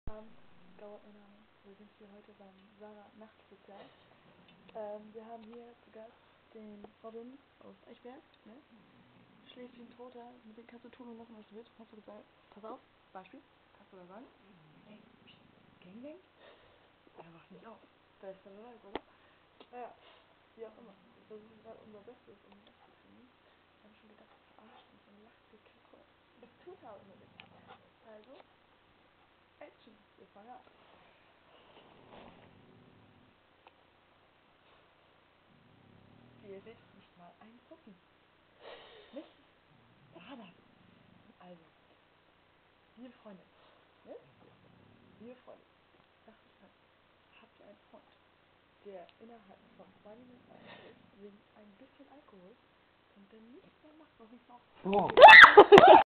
Willkommen bei Sari in der Nacht- Special
Das passiert wenn der Junge schon schläft die Mädelz aber noch viel zu fit sind xD und das um 7h morgens:D